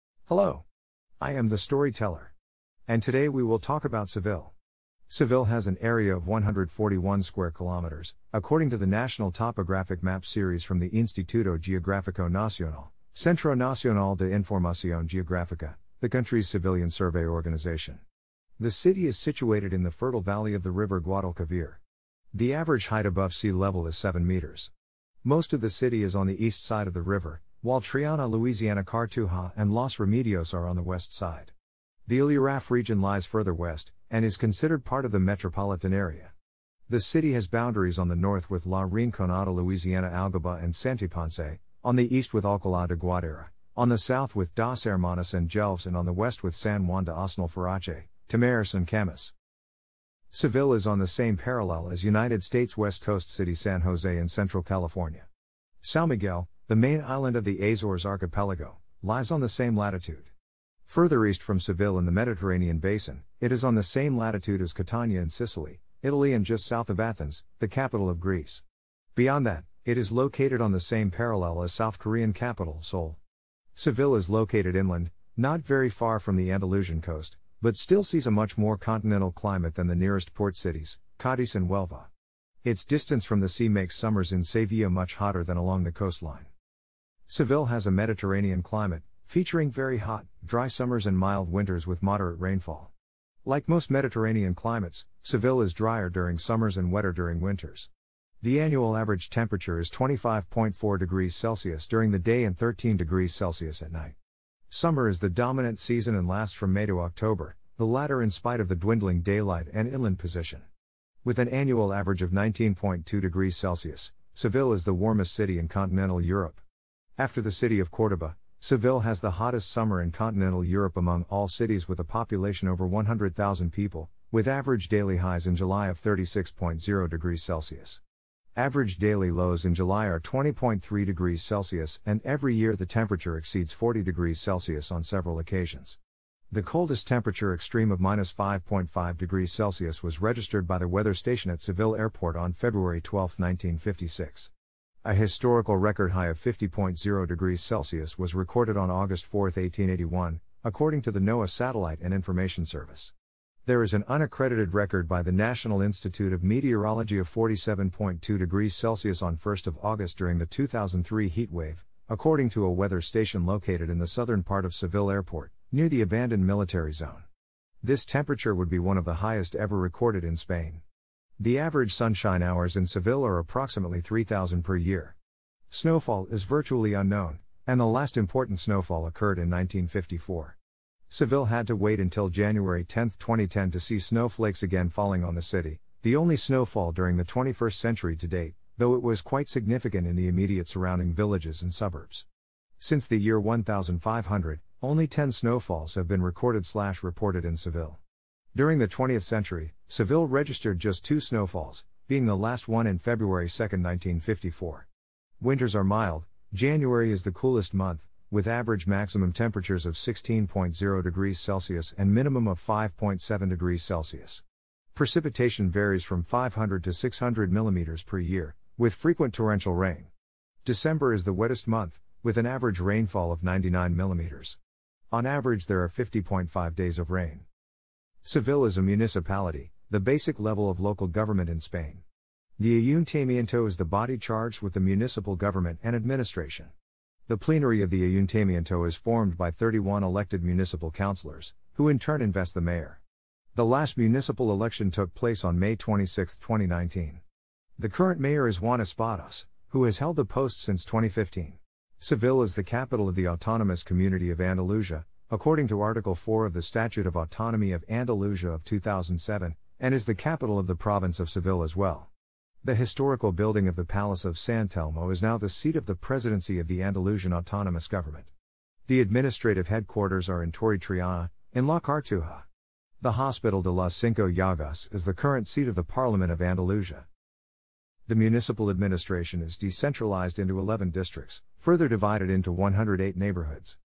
Seville - Geography, Climate and Government
Hello! I am the story teller... and today we will talk about Seville. Seville has an area of 141 km2 , according to the National Topographic Map  series from the Instituto Geográfico Nacional – Centro Nacional de Información Geográfica, the country's civilian survey organisation . The city is situated in the fertile valley of the River Guadalquivir. The average height above sea level is 7 metres .